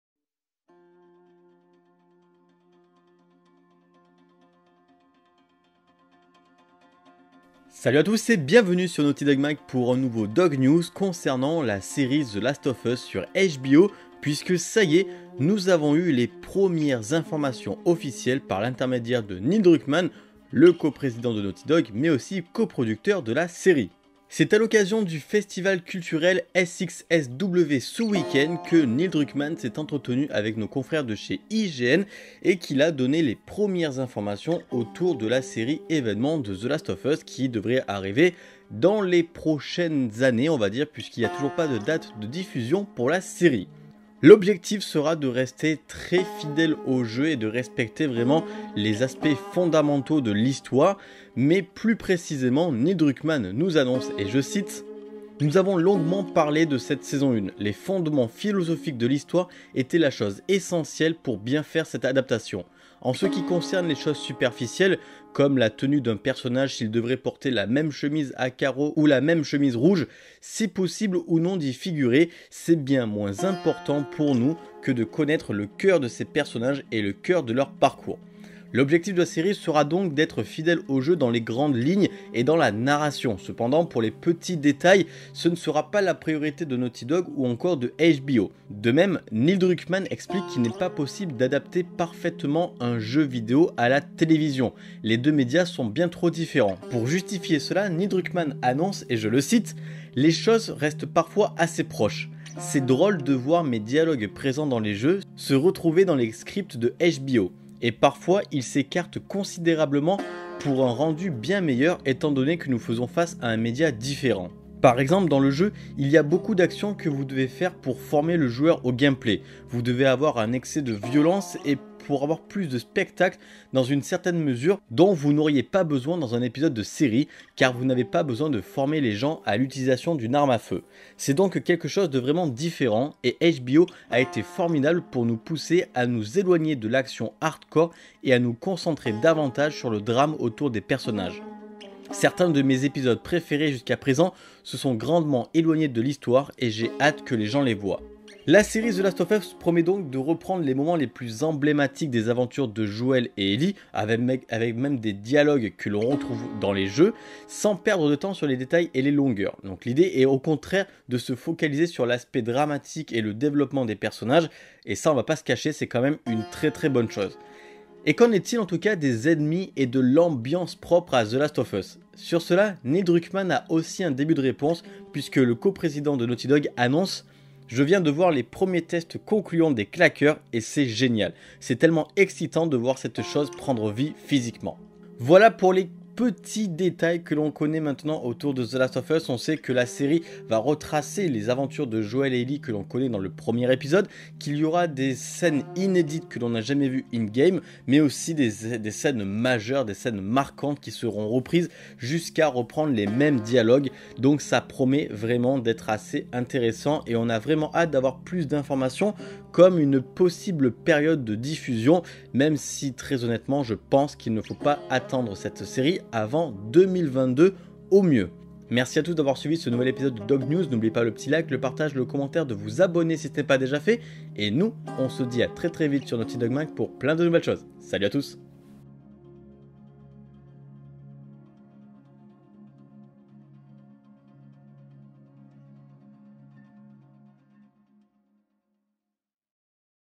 THE LAST OF US : PREMIÈRES INFOS OFFICIELLES SUR LA SÉRIE HBO !
Ce week-end, Neil Druckmann a dévoilé les premières informations officielles autour de la série The Last Of Us sur HBO à l'occasion du festival SXSW !

SUIVRE CHRIS :
_ Facebook :

ROCKSTAR MAG' :
_ Facebook :